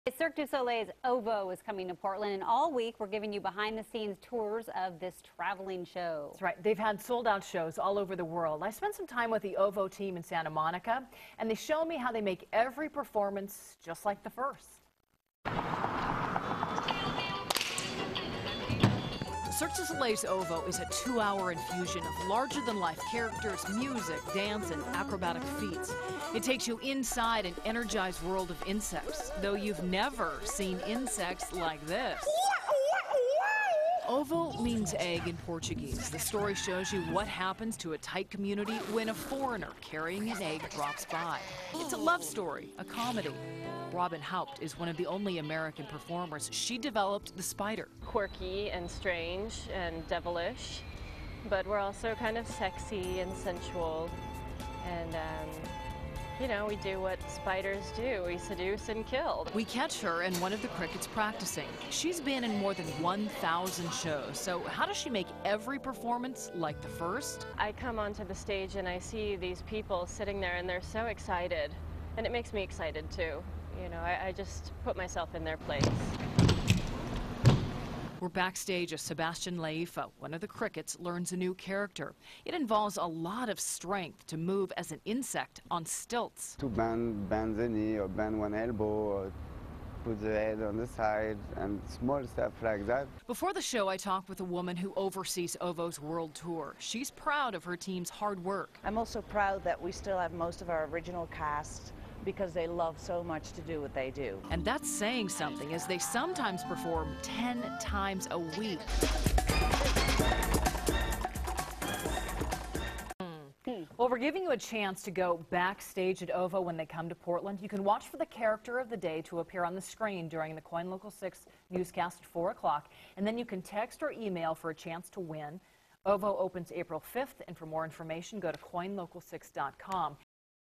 Cirque du Soleil, OVO
All week KOIN's Amy Troy  is going behind the scenes of this traveling show.